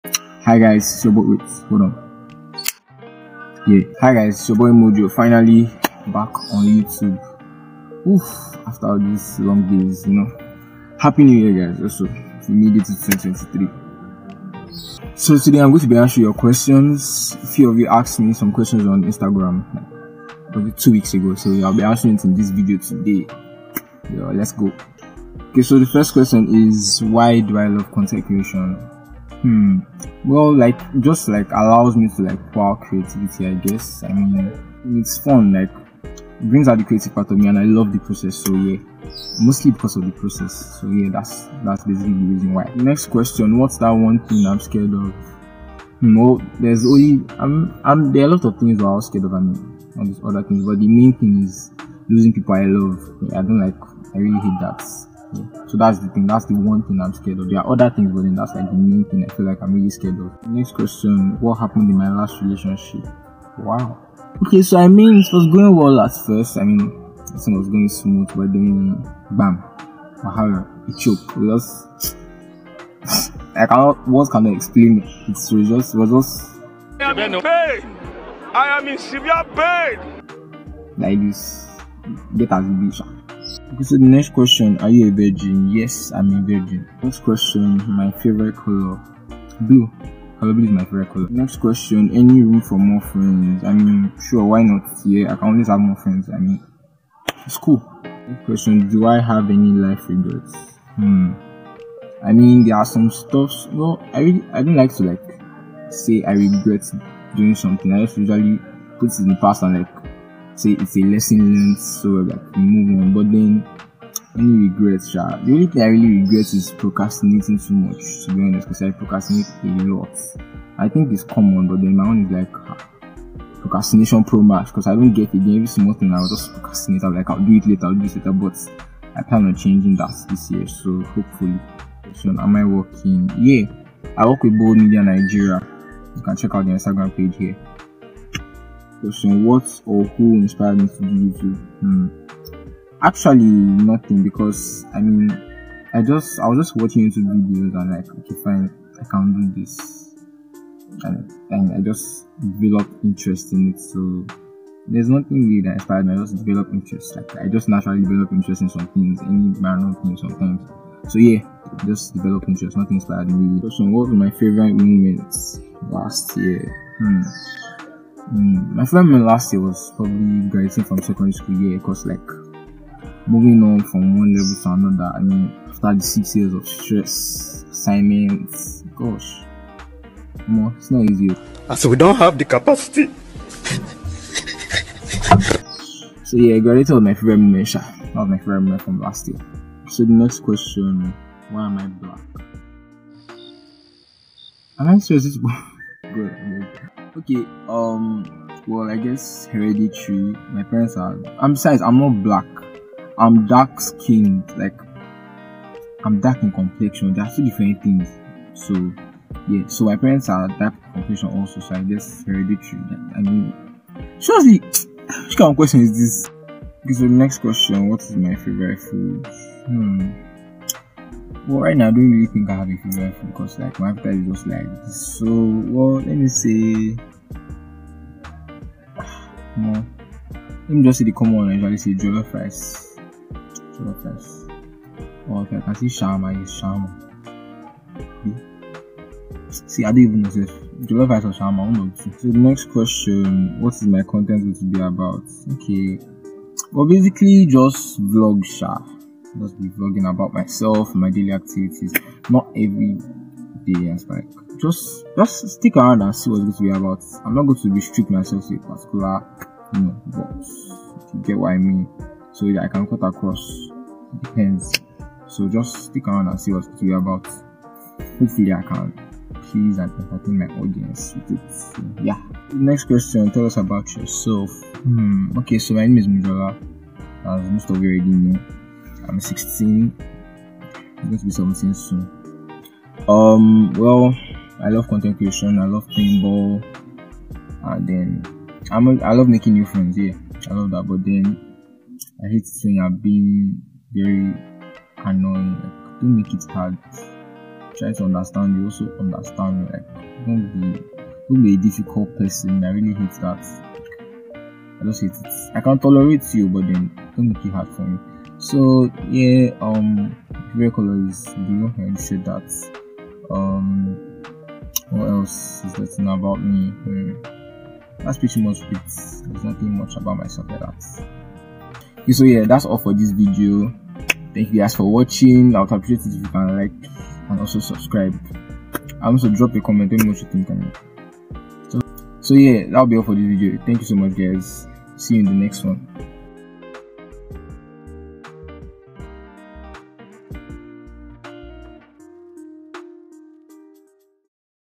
✨Q&A Session/My first video✨qandasession
In this video i'll be answering the questions some of you asked on Instagram. I'm here to deliver content so just sit back and watch me do my thing.